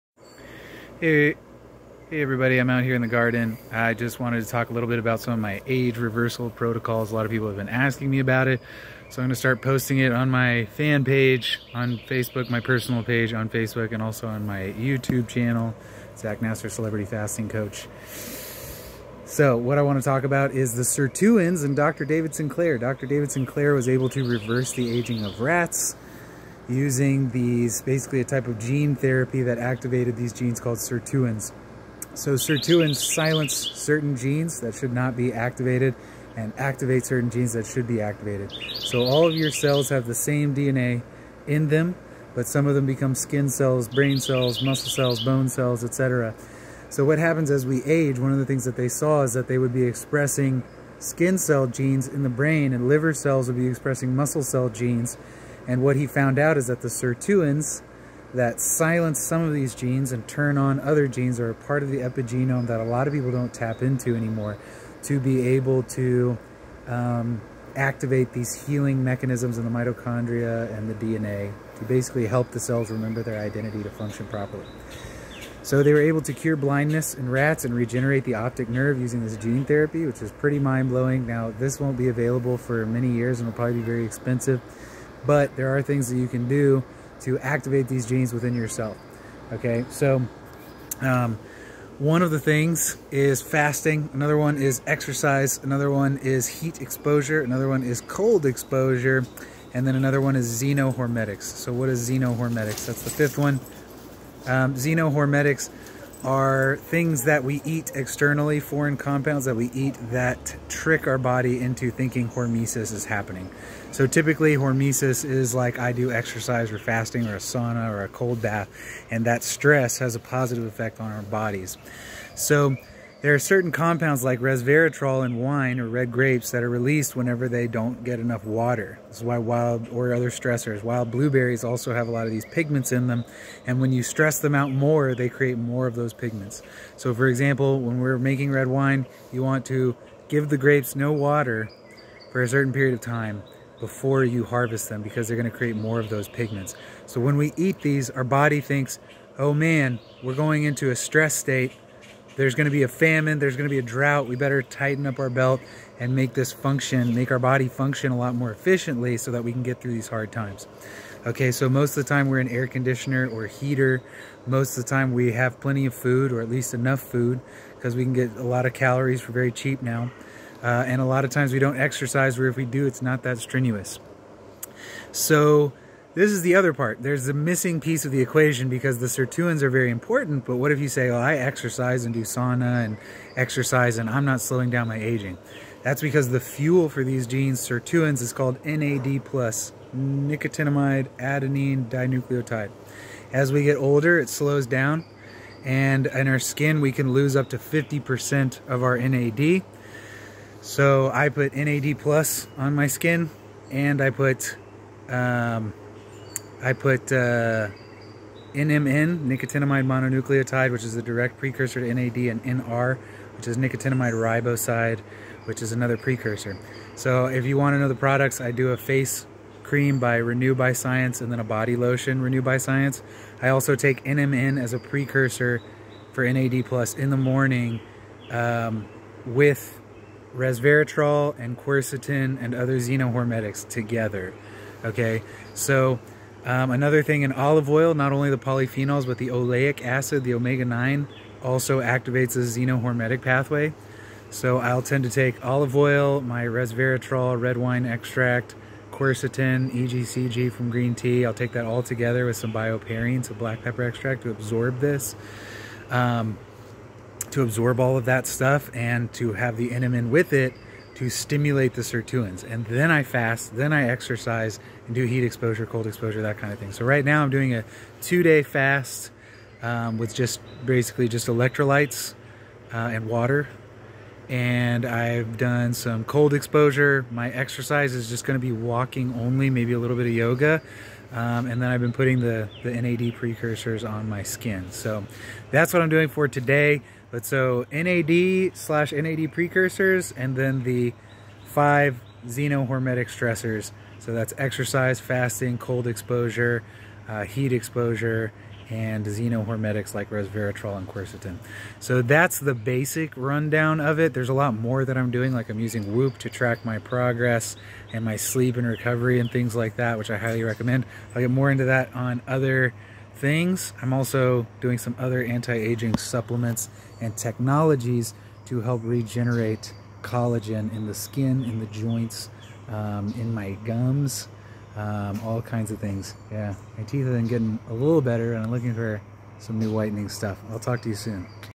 How to Activate and Fuel Sirtuins for Age Reversal
This is a really basic overview of how to leverage the sirtuins and the discoveries of Dr. David Sinclair et. al. for age reversal and rejuvenation of the body on a cellular level.